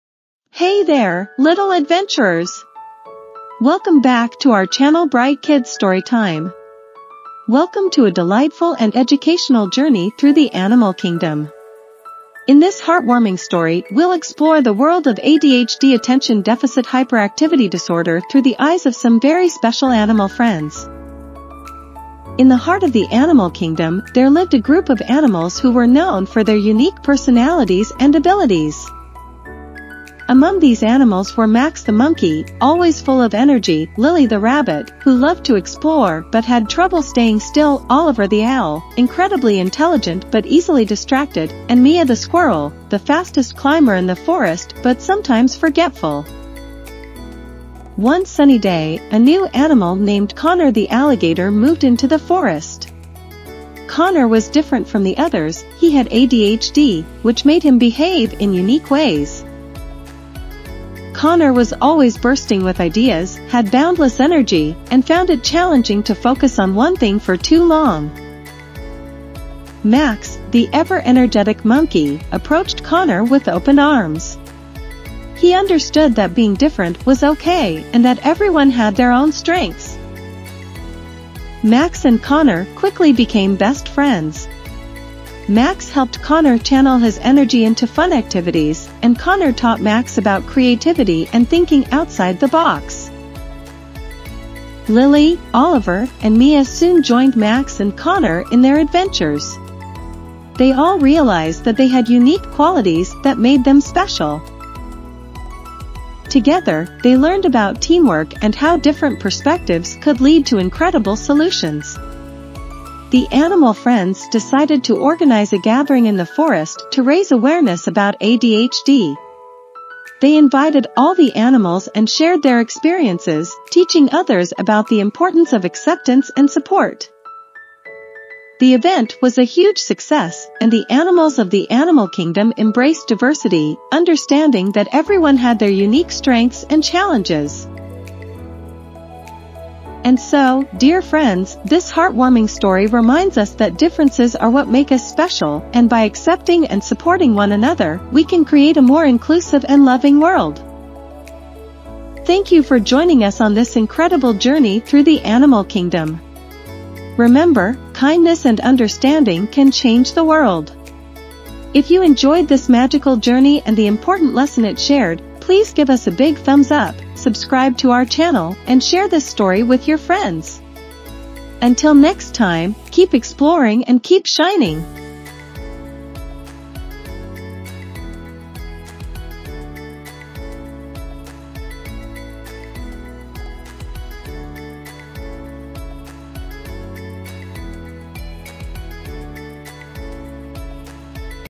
ADHD in the Animal Kingdom Embracing Differences. اضطراب فرط الحركة ونقص الانتباه في مملكة الحيوان.
🌟✨ "Adventures in the Animal Kingdom: Embracing Differences!" 🐒🐰🦉🐿️🐊

Join Max the Monkey, Lily the Rabbit, Oliver the Owl, Mia the Squirrel, and Conner the Alligator in an extraordinary journey through the Animal Kingdom as they discover the wonders of ADHD. 🌳🐾